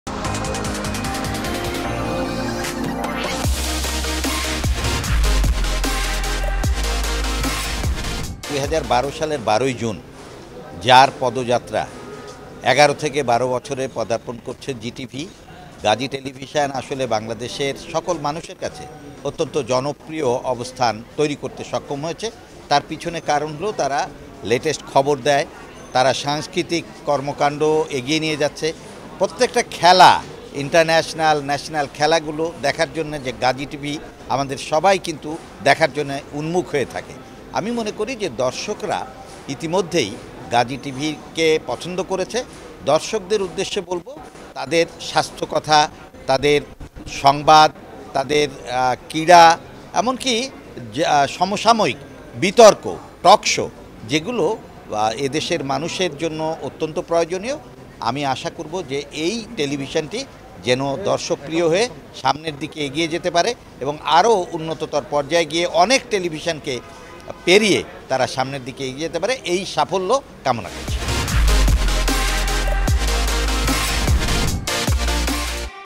জিটিভির ১২ তম বর্ষে পদার্পণ ও প্রতিষ্ঠা বার্ষিকী উপলক্ষে বিশিষ্টজনদের শুভেচ্ছা বাণী | GTV BIRTHDAY
জিটিভির ১২ তম বর্ষে পদার্পণ ও প্রতিষ্ঠা বার্ষিকী উপলক্ষে বিশিষ্টজনদের শুভেচ্ছা বাণী | GTV BIRTHDAY | 12June2023

জিটিভির প্রতিষ্ঠা বার্ষিকী উপলক্ষে শুভেচ্ছা বাণী:
শারফুদ্দিন আহমেদ
উপাচার্য, বিএসএমএমউই